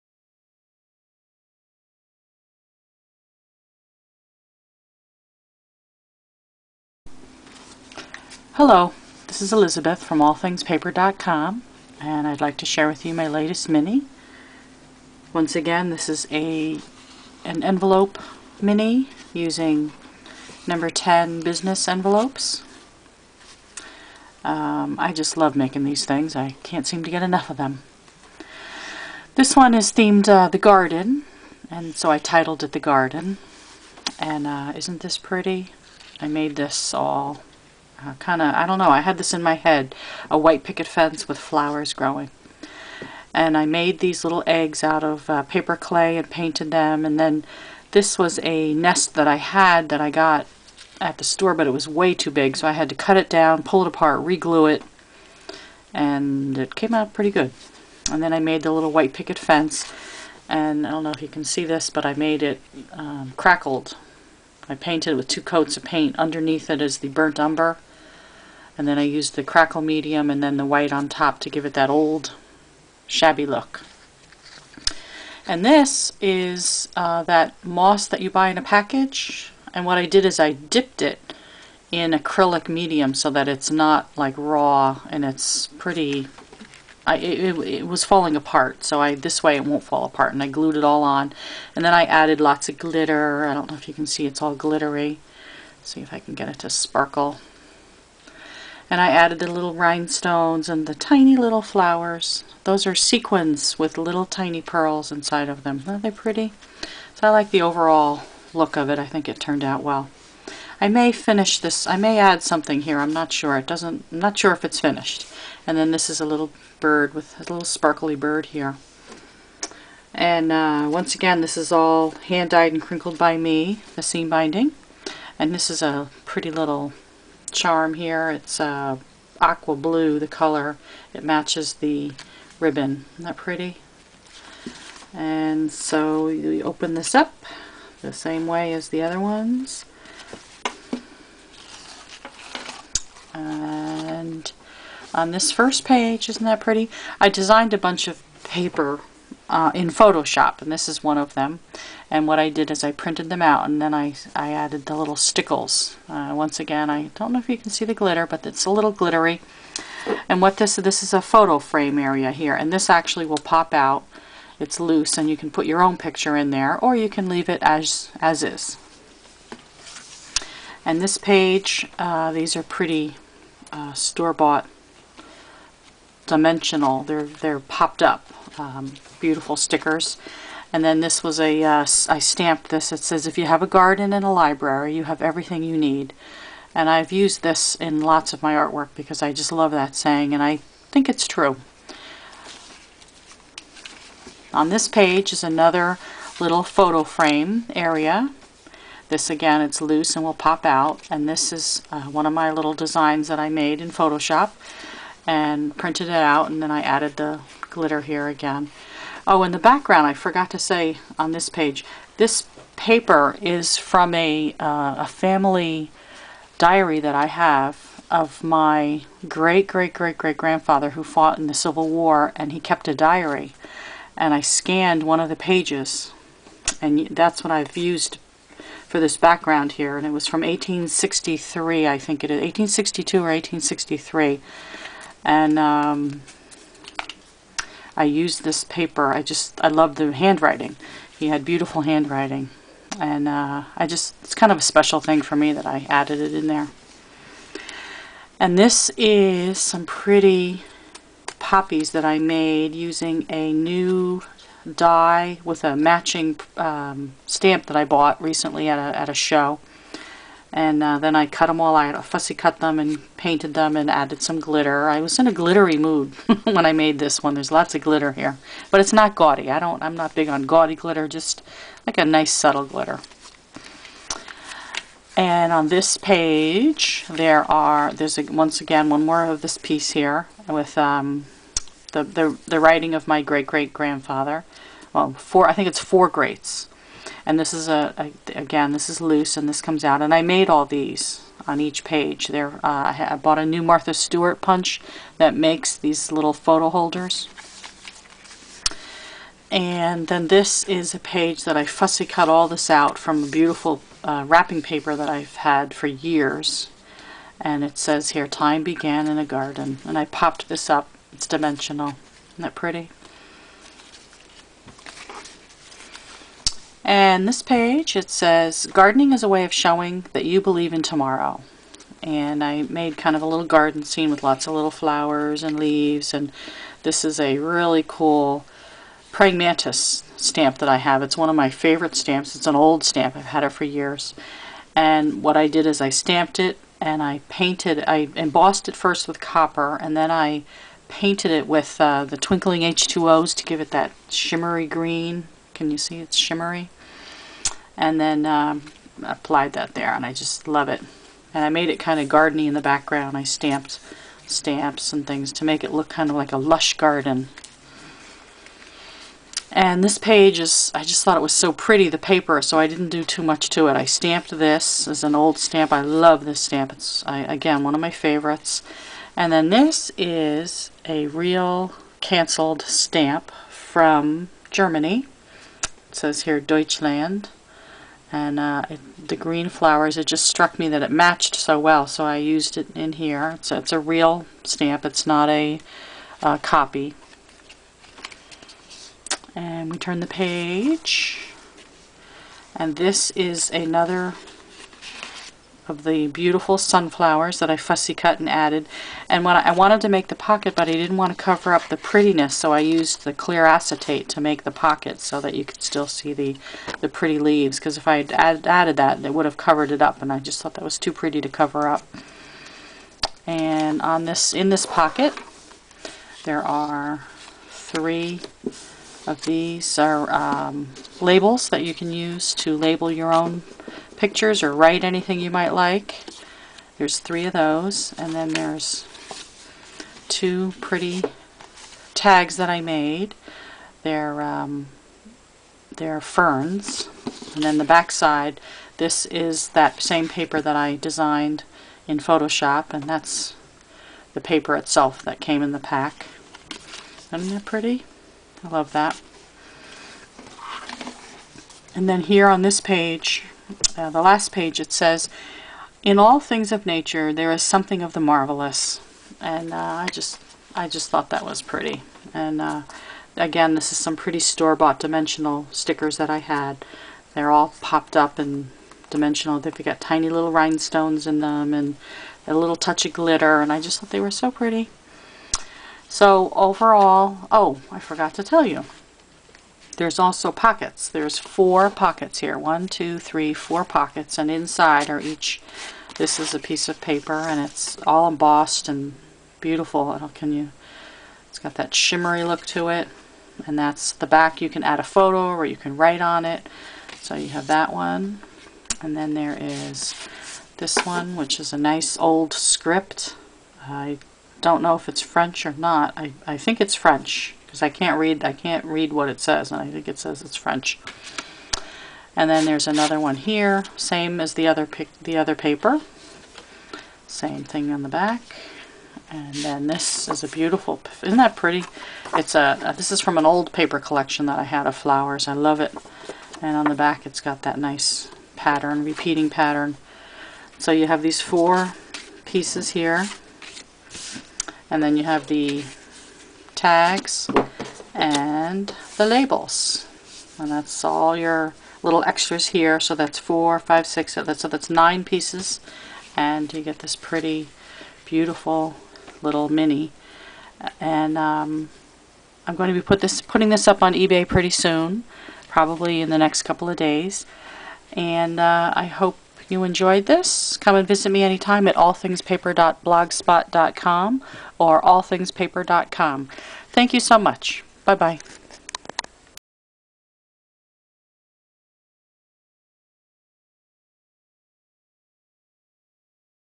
Bird's Nest Flower Garden Envelope Mini Scrapbook
12 page mini I made using #10 business envelopes.